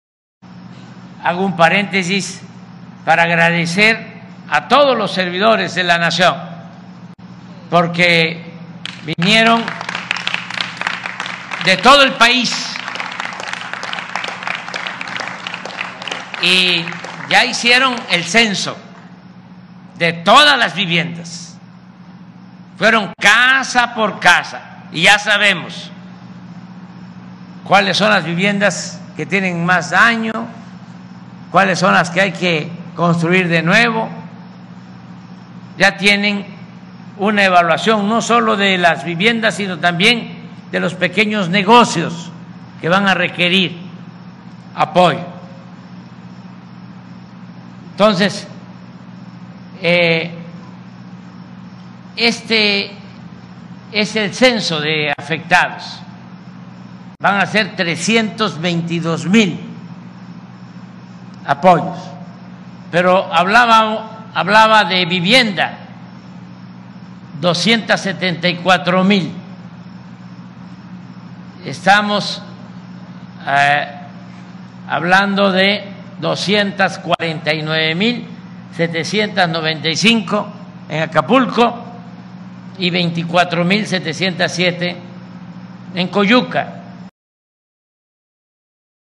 AMLO ANUNCIA PLAN DE RECONSTRUCCIÓN EN ACAPULCO
El próximo miércoles inicia la dispersión de los 8 mil pesos para la limpieza de Acapulco, Guerrero, y así será la dispersión de apoyos directos de 35 mil a 60 mil pesos para el apoyo de viviendas